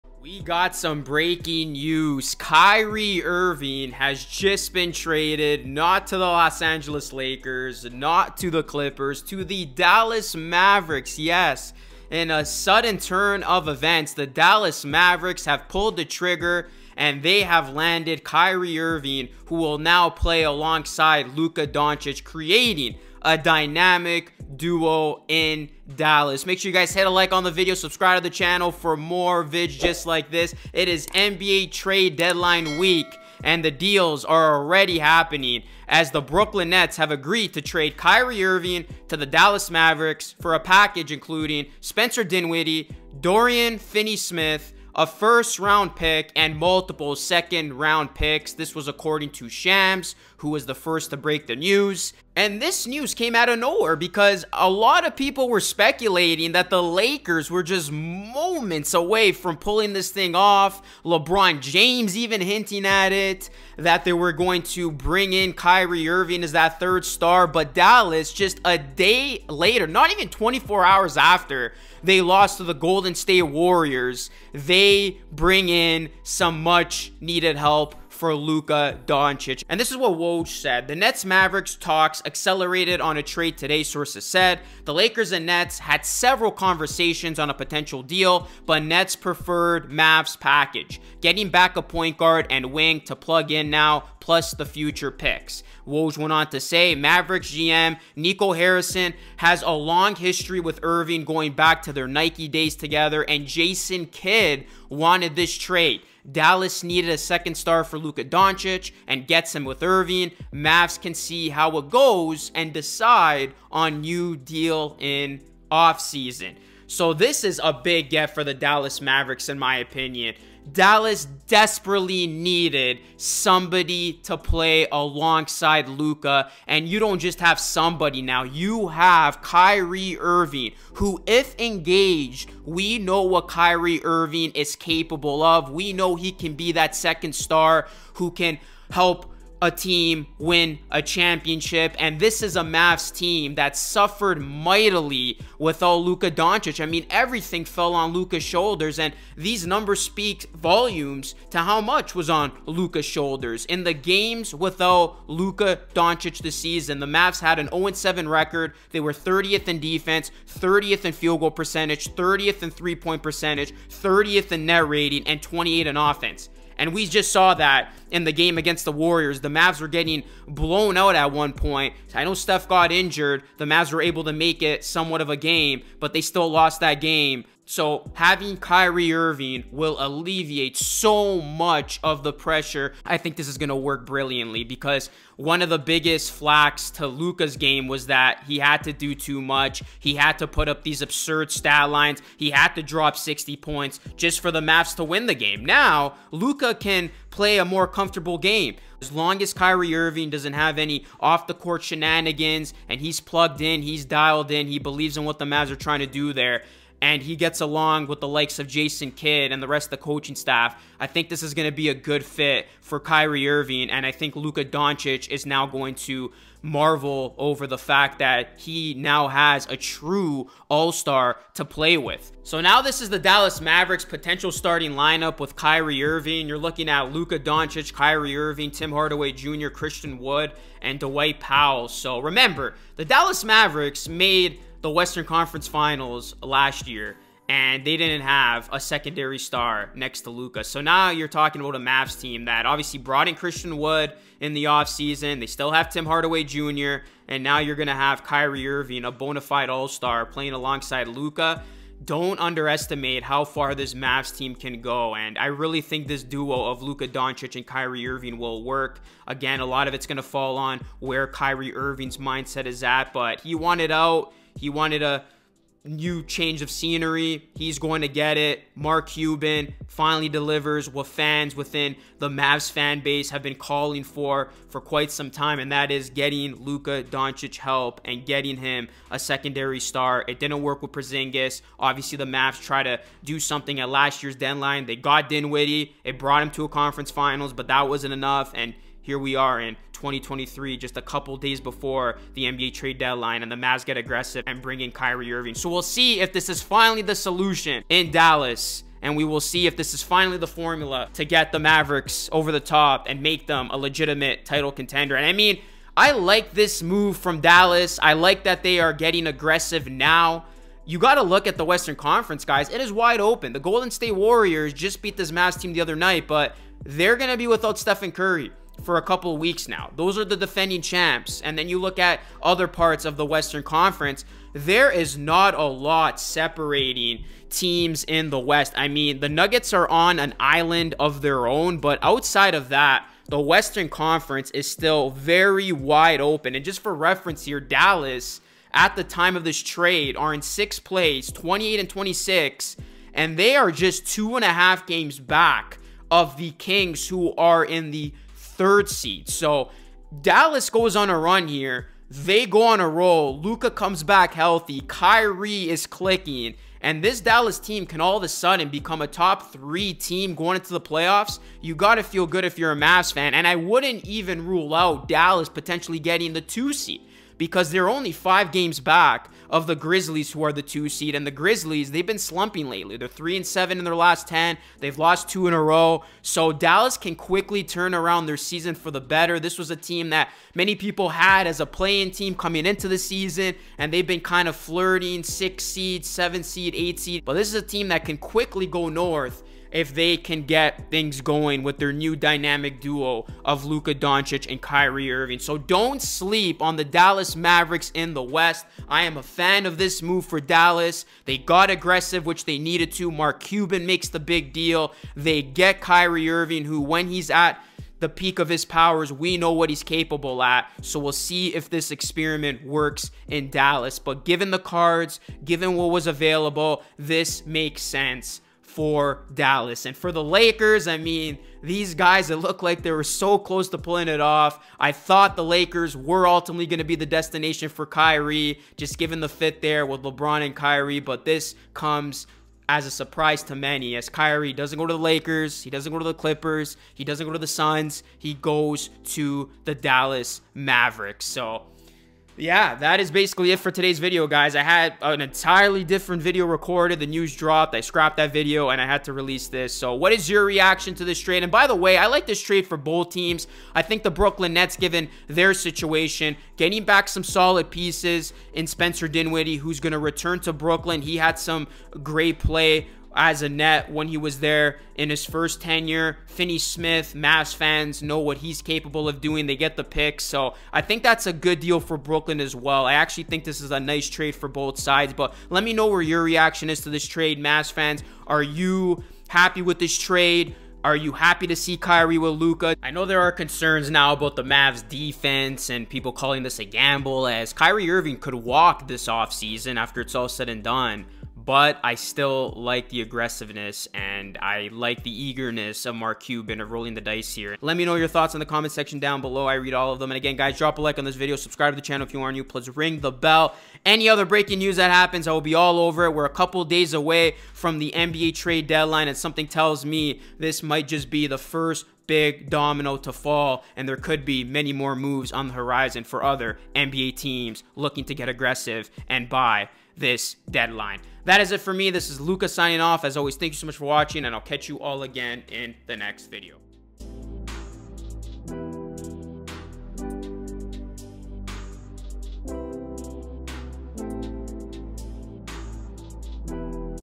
Dallas Mavericks TRADE For Kyrie Irving Reaction
Luka Doncic finally has his co-star.